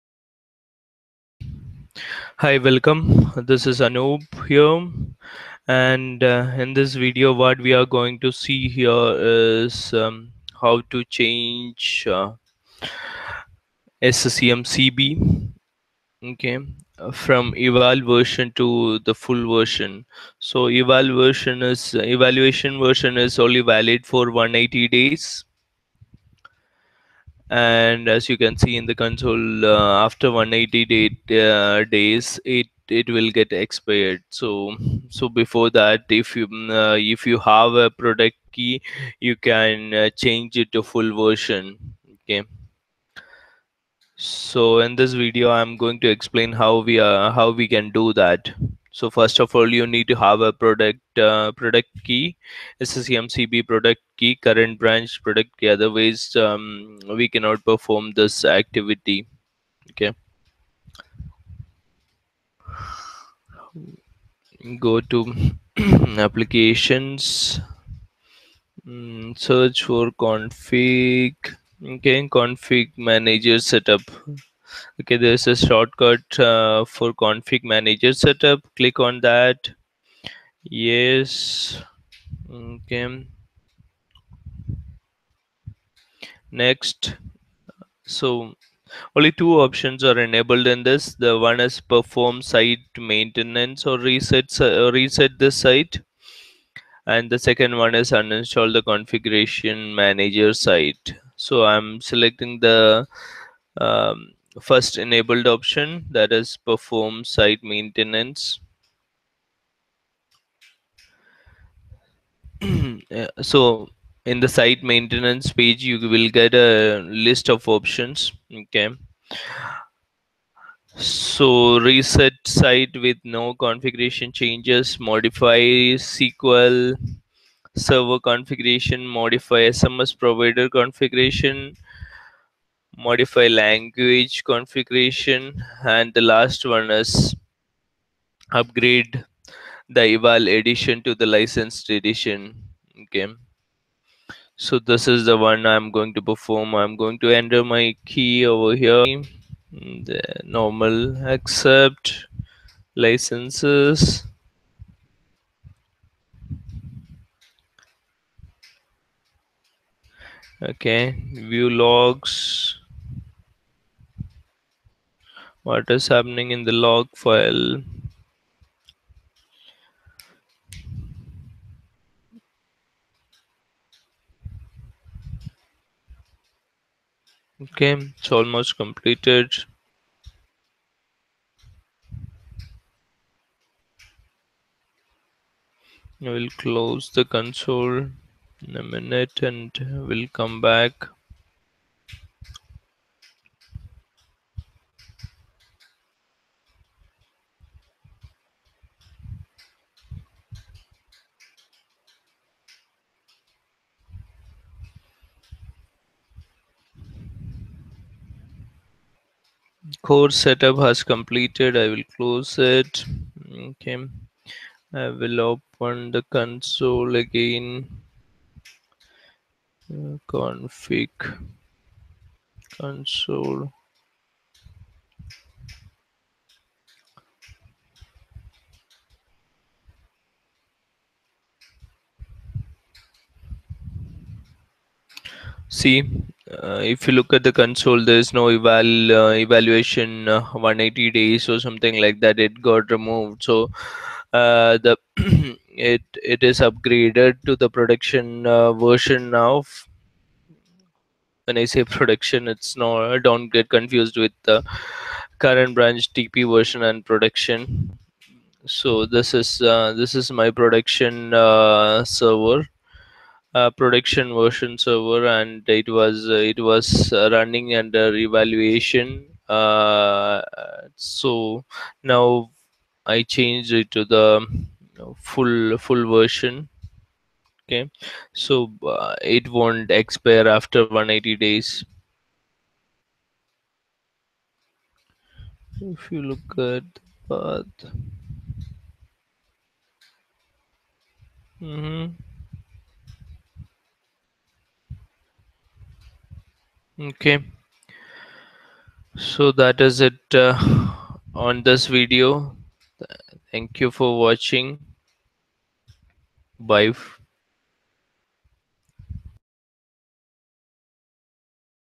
How to Change SCCM CB from Evaluation Version to Full Version
I'm using SCCM Current Branch (CB) 1606  CAS server to perform this activity. We have to covert all the site servers (CAS, Child Primaries and Stand-alone primary servers) using the same method.  More details -